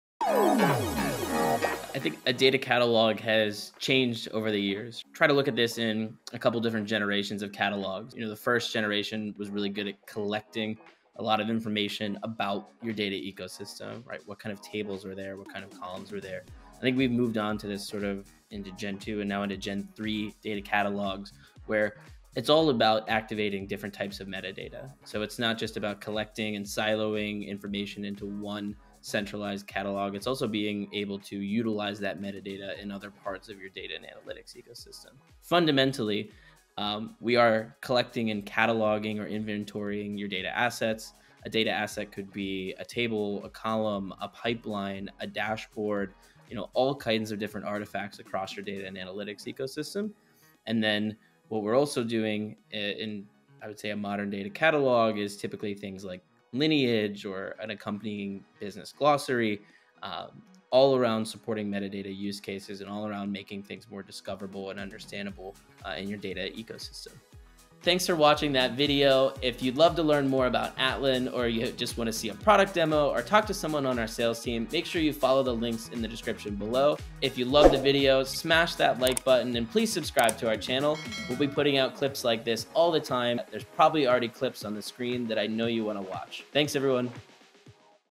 Data Catalogs Explained in 90 Seconds: Generations 1→3 & Modern Evolution
A data catalog is a centralized system that inventories and organizes metadata across your data ecosystem — including tables, columns, dashboards, pipelines, and business metrics. In this 90-second explainer, Austin Kronz, Director of Data Strategy at Atlan, breaks down what a data catalog is, how it has evolved over generations, and why modern teams rely on active metadata to power discovery, lineage, and trust.

Learn how data catalogs have progressed from simple metadata collectors to modern platforms that activate metadata across BI tools, workflows, and daily decision-making.

Chapters
00:01 How Data Catalogs Have Changed
00:09 Generation 1 → 2 → 3: The Evolution of Catalogs
00:22 What a Modern Data Catalog Actually Does
00:35 Lineage, Business Glossaries & Key Metadata Use Cases
01:00 The Role of Active Metadata in Today’s Ecosystem
01:08 Summary & Next Steps

Key Takeaways
➡️ A data catalog organizes and inventories metadata across tables, columns, dashboards, and pipelines.
➡️ Catalogs have evolved from collecting metadata to activating metadata across an organization’s tools and workflows.
➡️ Modern catalogs support lineage, business glossaries, data quality signals, and other key metadata use cases.
➡️ Active metadata platforms push context back into BI tools, Slack, Jira, and other systems where teams work.
➡️ Generations 1–3 show the shift from siloed metadata → integrated context → automated, AI-powered metadata systems.
➡️ Modern metadata platforms improve discoverability, trust, and understanding across the data ecosystem.